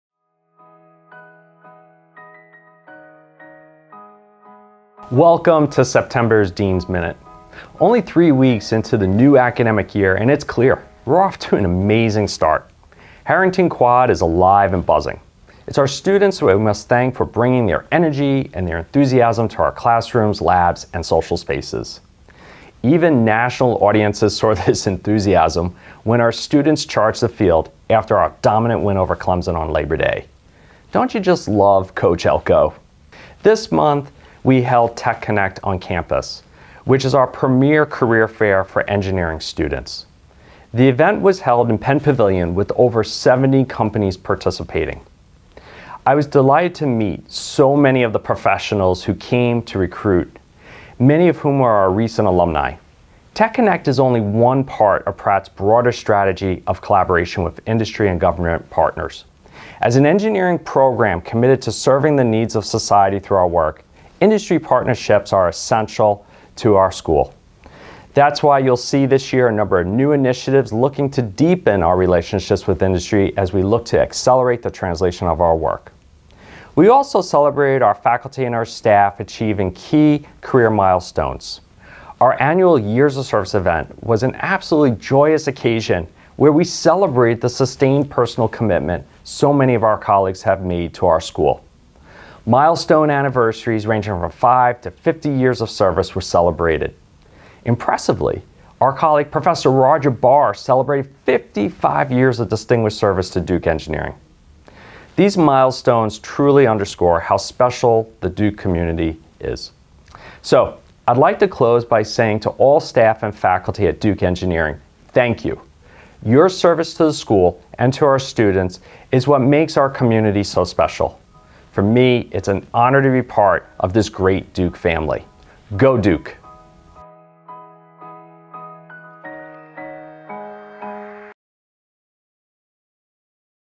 Pratt Blueprint: September 2023 Edition
Dean Jerome Lynch welcomes the Duke Engineering community to the September 2023 edition of "Pratt Blueprint," a monthly newsletter curated with purpose: to provide high-impact information about our community in a useful and digestible format.

For more information on the stories mentioned, visit:
Research With Impact

Meet Our Newest Faculty Members:
Duke Engineering is welcoming 14 new faculty members to its ranks this calendar year. Discover how their diverse expertise and backgrounds are reinforcing our mission to serve society.

The Duke Difference
Spending the Summer Fighting Heat Islands
Duke’s Climate+ program tasks teams of students with tackling real-world issues over the summer. Check out how one team is using data to mitigate hotspots in downtown Durham.

Building Community

Expanding the Future Faculty Pipeline

Not every PhD and postdoc has access to the support needed to compile a stellar faculty application. Learn how Duke Engineering is leveling the playing field by de-mystifying the process.

From rainforest travels to lab course facilitation and design, senior BME lab administrator Margaret (Maggie) Gatongi never passes up an opportunity to learn. Read all about how her passion for exploring is leading to outstanding learning experiences for Duke Engineers.